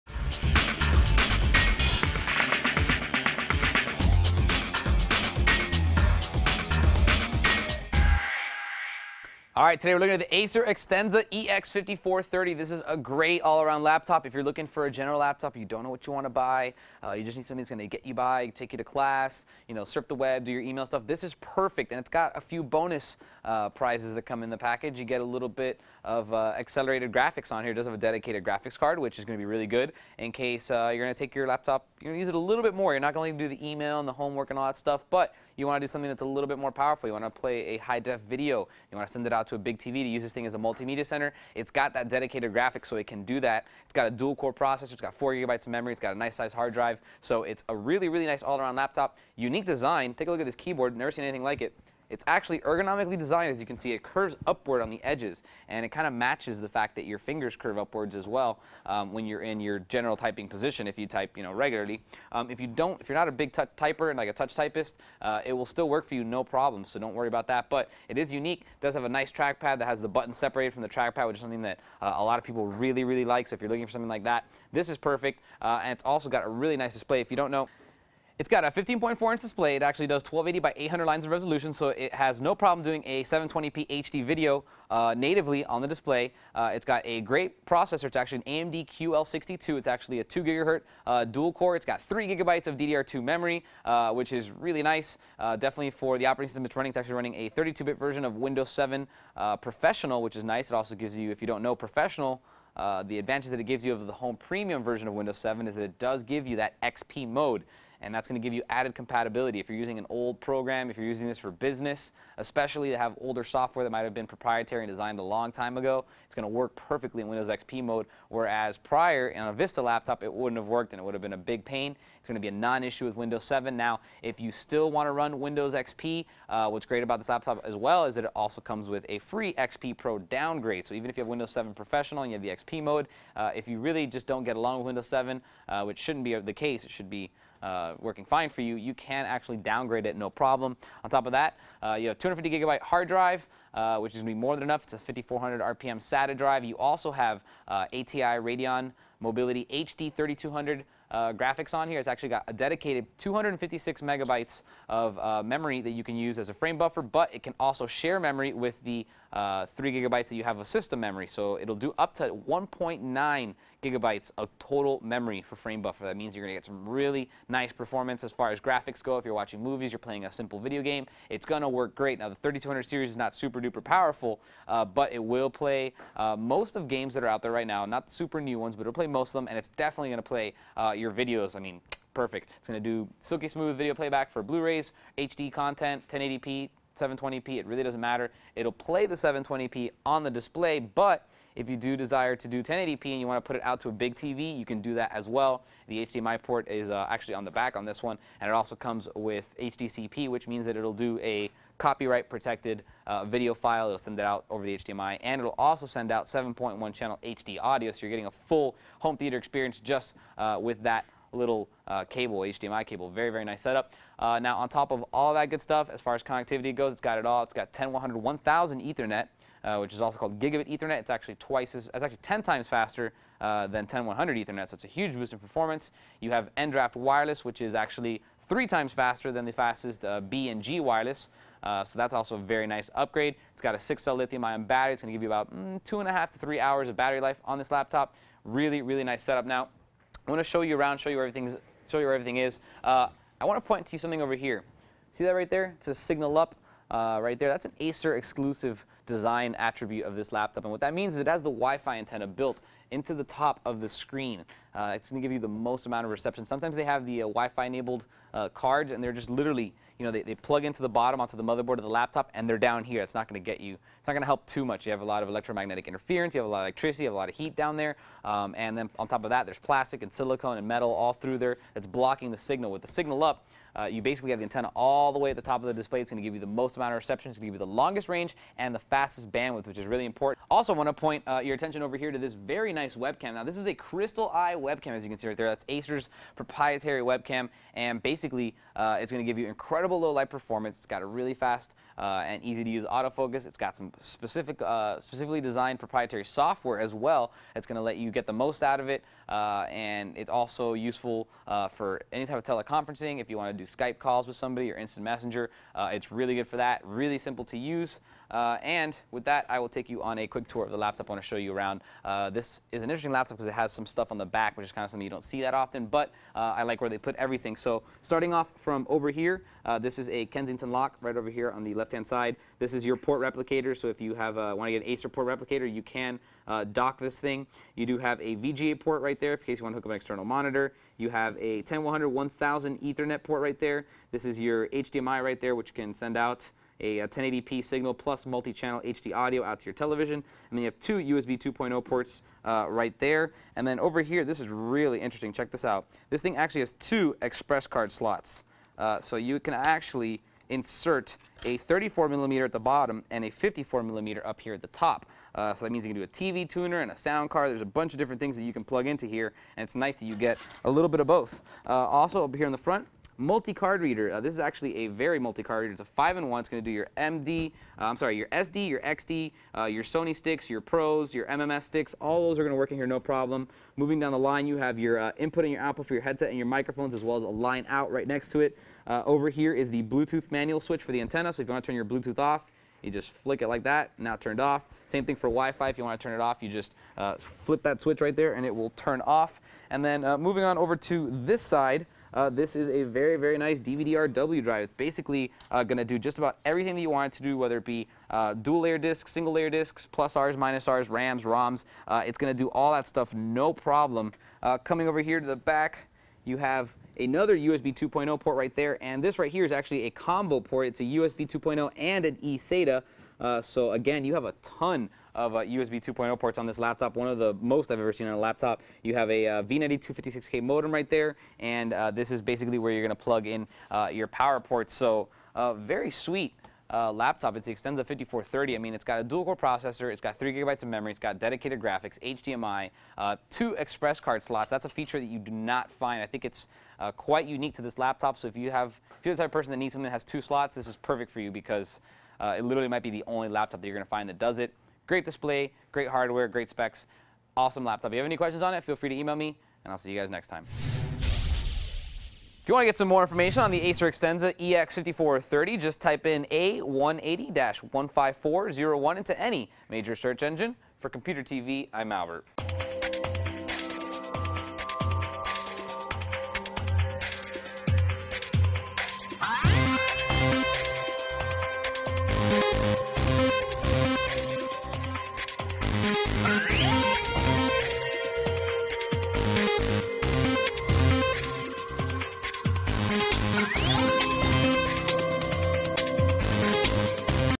Acer Extensa EX5430-6670 Notebook
The Acer Extensa EX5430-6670 Notebook PC is perfect for media, productivity and communication on the move and comes running Windows 7 Professional 32-bit operating system. The Acer Extensa EX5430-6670 Notebook PC is powered by an AMD Athlon X2 QL-62 2.0GHz Dual Core Processor and 3GB of DDR2 memory. The 15.4-inch diagonal WXGA widescreen display is capable of 1280 x 800 resolution augmented by an ATI Radeon HD 3200 Graphics solution with 256MB dedicated system memory and up to 1663MB shared system memory. Watch your favorite HD media and content on the Acer Extensa EX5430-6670 Notebook PC, or output to any HD-ready large format display via the onboard HDMI port. Store your software, data, documents and more on the 250GB hard drive and watch (or burn) your favorite DVDs with the Super-Multi Optical Drive. The Acer Extensa EX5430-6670 Notebook PC also has 802.11a/b/g/n integrated wireless, USB 2.0 and audio inputs, and even eSATA connectivity for your external peripherals. The Acer Extensa EX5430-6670 Notebook PC is an excellent notebook perfectly suited for your communication, work, or personal entertainment on the road.